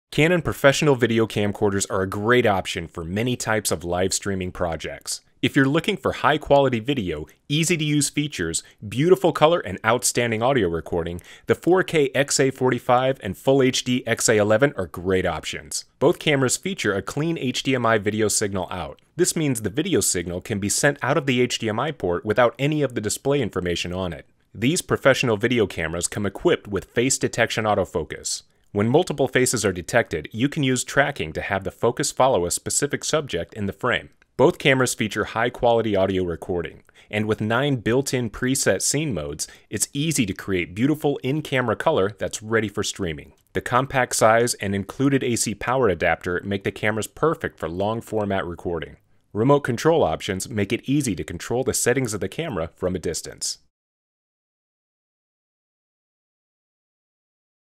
Streaming with Canon's XA45 and XA11 Camcorders
Canon's Professional Camcorders are a great option for many types of live streaming productions. The 4K XA45 and HD XA11 provide beautiful imagery at a competitive price point, and can easily be configured for streaming live over IP.